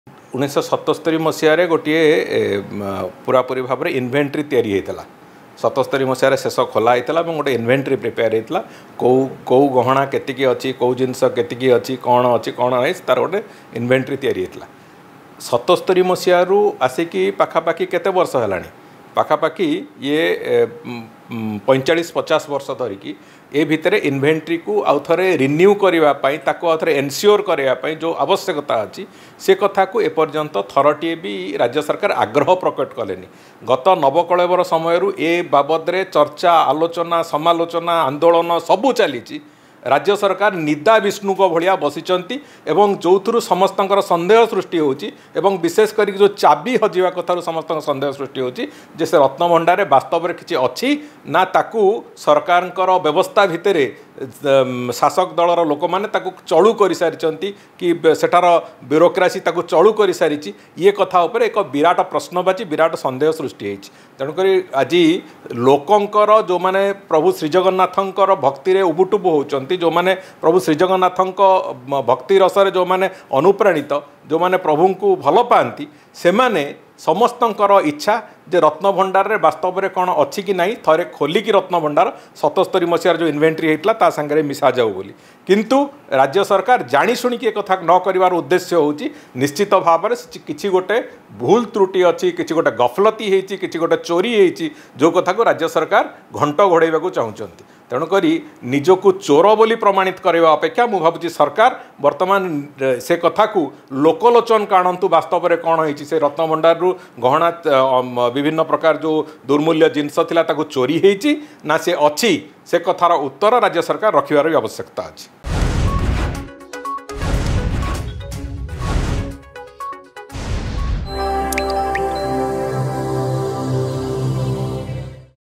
Something Is Fishy As Odisha Govt Is Not Opening Ratna Bhandar Of Shrmandir: Prithiviraj Harichandan
Something Is Fishy As Odisha Govt Is Not Opening Ratna Bhandar Of Shrmandir: Pruthwiraj Harichandan.